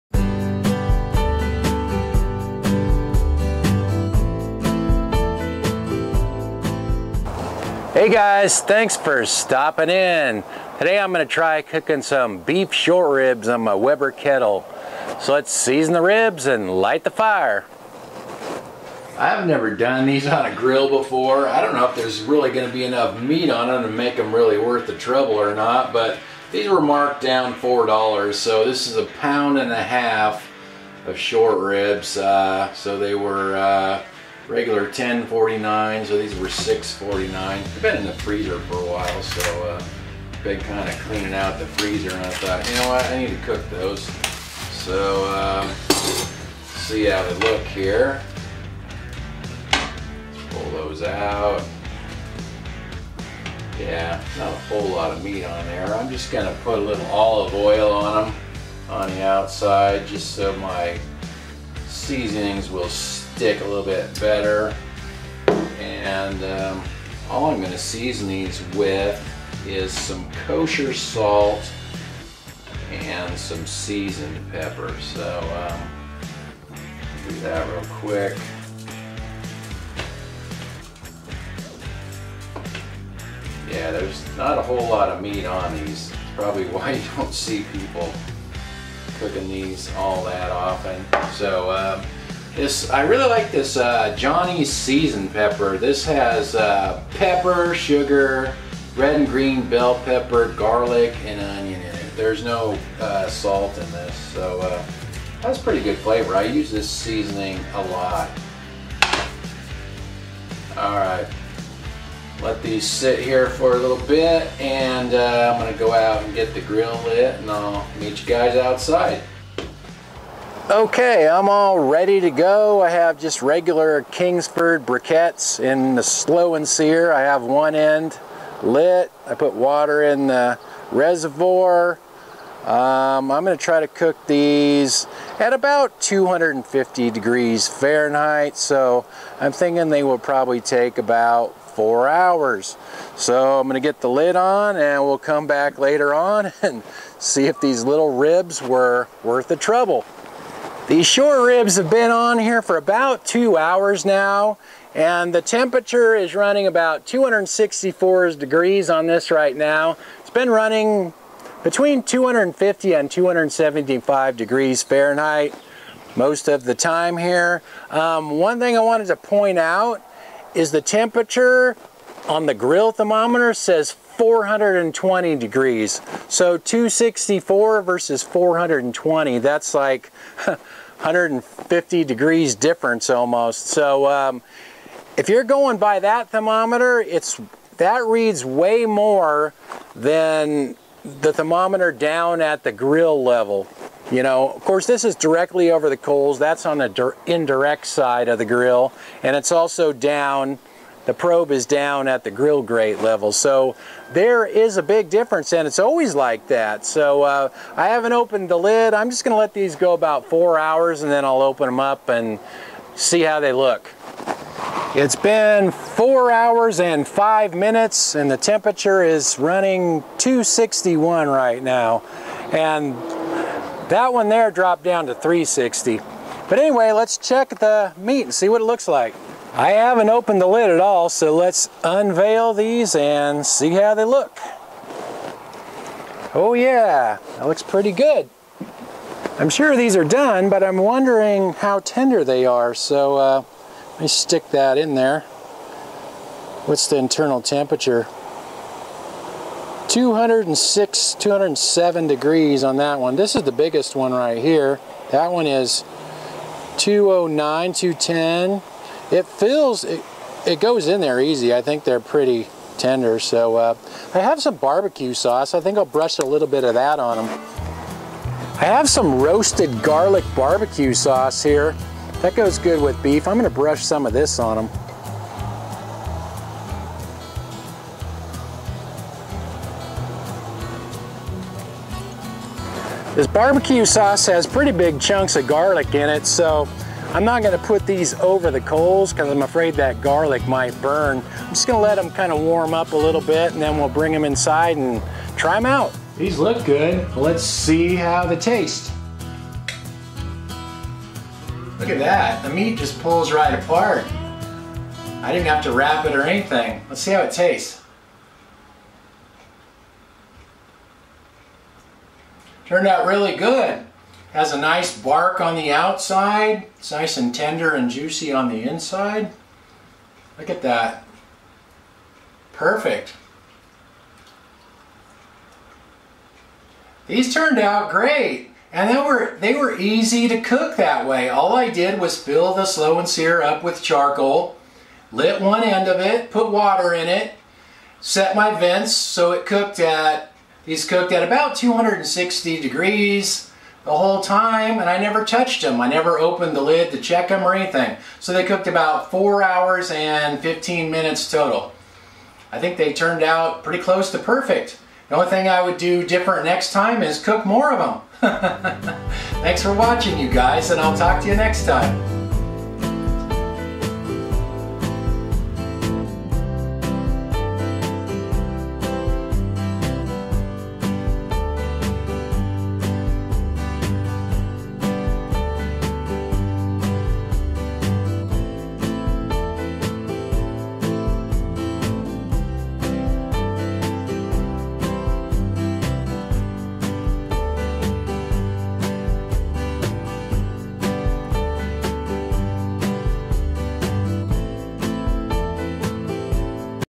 BBQ Short Ribs on the Weber Kettle with the Slow 'N Sear
In this video, I BBQ some beef short ribs on my Weber Kettle charcoal grill using the Slow 'N Sear BBQ accessory. The cooking time was about 4 hours on a 250-275 degree grill.